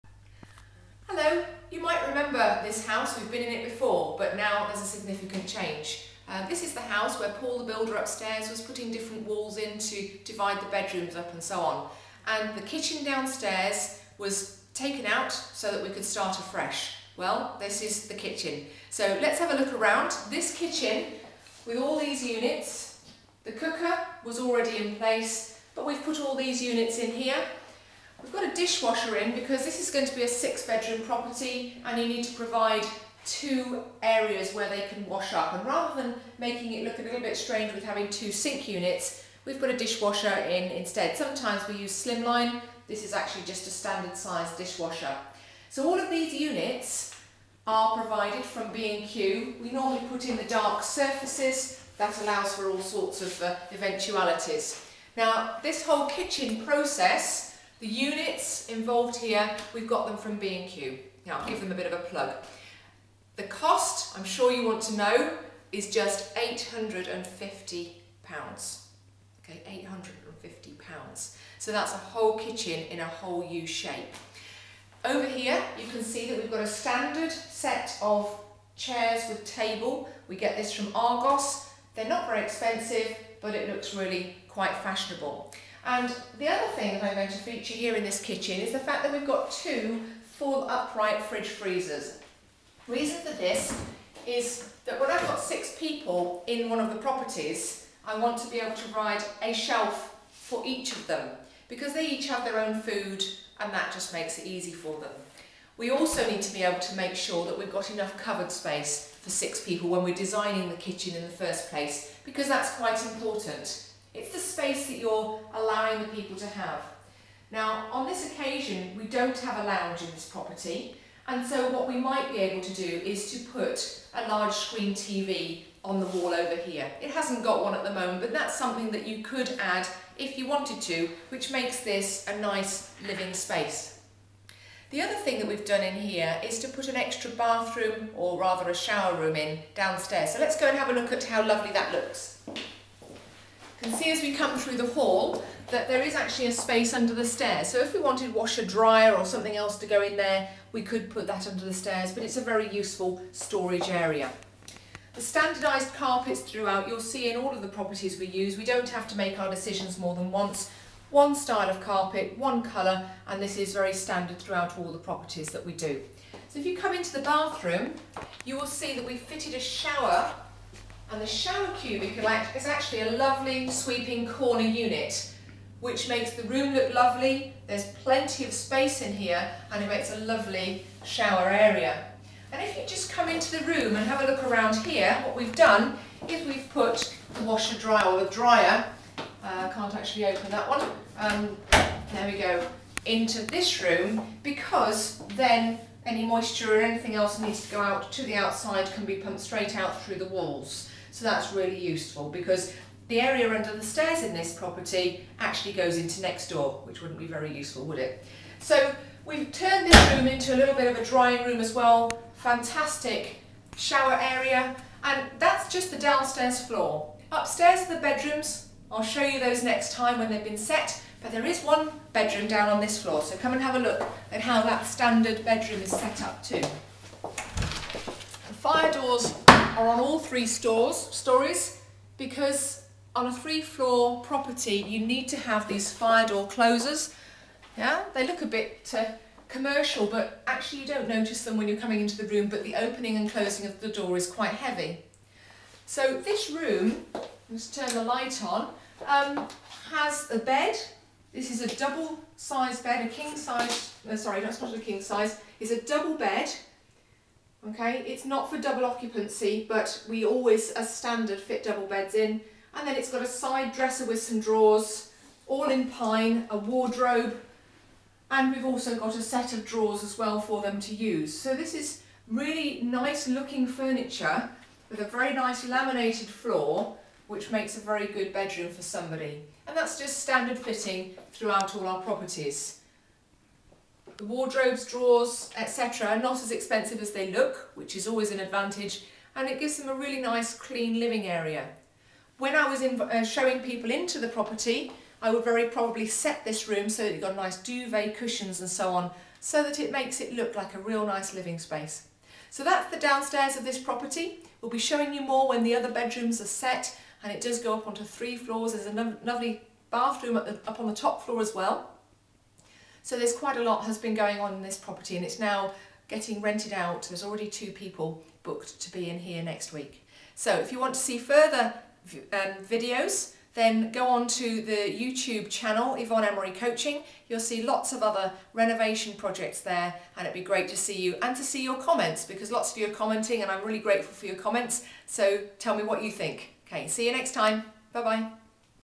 A beautiful kitchen for just £850.AVI
A kitchen was ripped out and we started again from scratch.  Often people ask how much it costs - this one cost just £850 from B&Q.  The units are standard.  The utilities are extra.  It looks great and all on a budget.